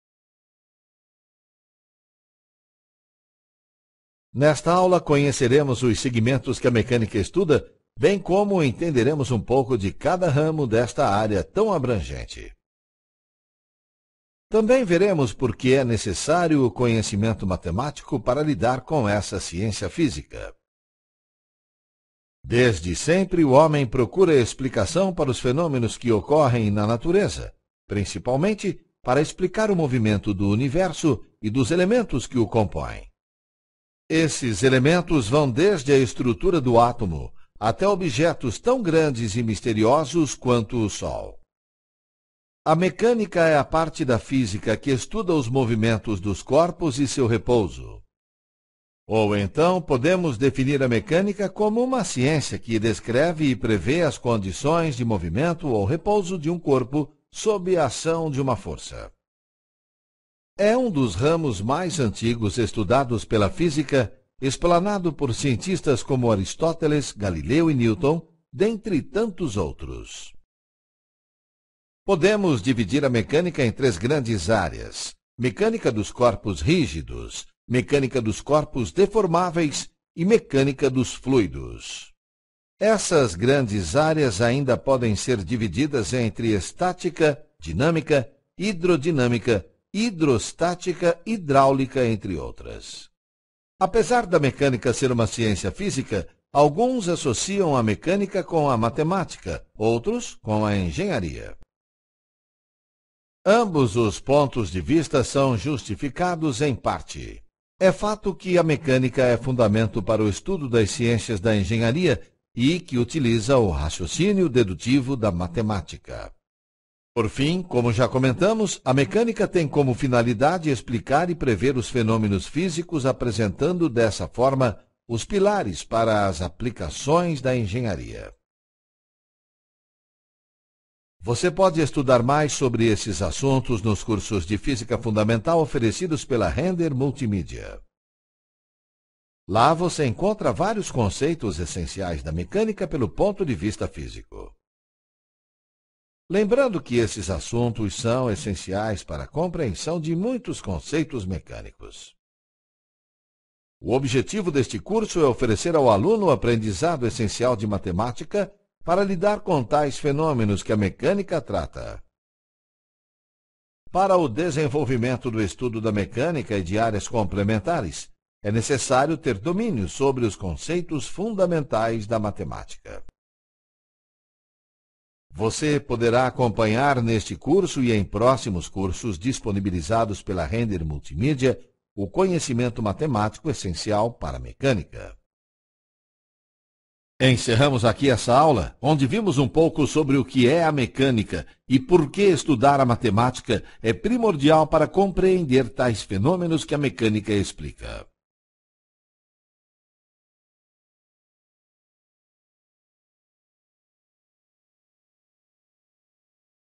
Aula 2 - Matemática Essencial para Mecânica
Nesta aula conheceremos os segmentos que a mecânica estuda, bem como entenderemos um pouco de cada ramo desta área tão abrangente. Também veremos porque é necessário o conhecimento matemático para lidar com essa ciência física.